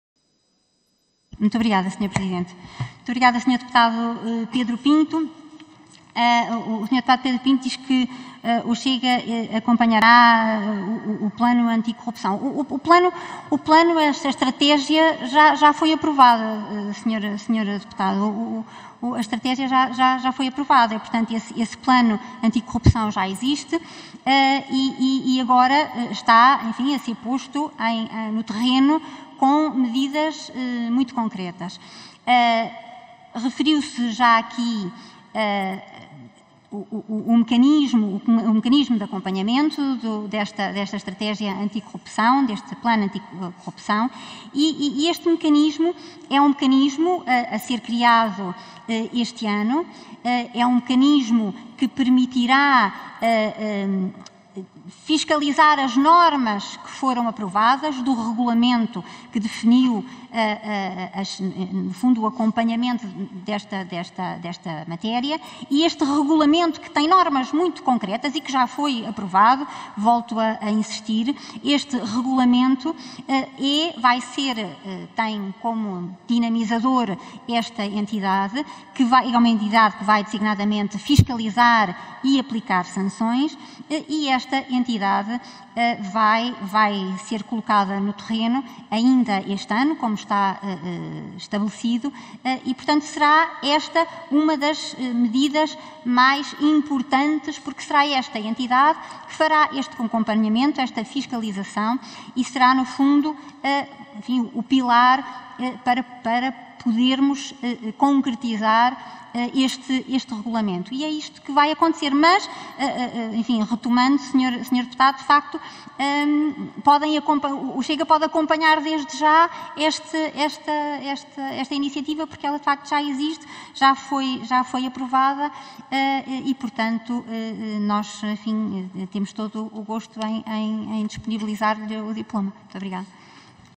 04-05-2022 | OE2022 - Resposta da Ministra da Justiça ao Deputado Pedro Pinto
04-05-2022 | Resposta da Ministra da Justiça, Catarina Sarmento e Castro, ao Deputado Pedro Pinto (CH), em audição da apreciação, na especialidade, da Proposta de Lei (GOV) - "Aprova o Orçamento do Estado para 2022” - Comissão de Orçamento e Finanças - Comissão de Assuntos Constitucionais, Direitos Liberdades e Garantias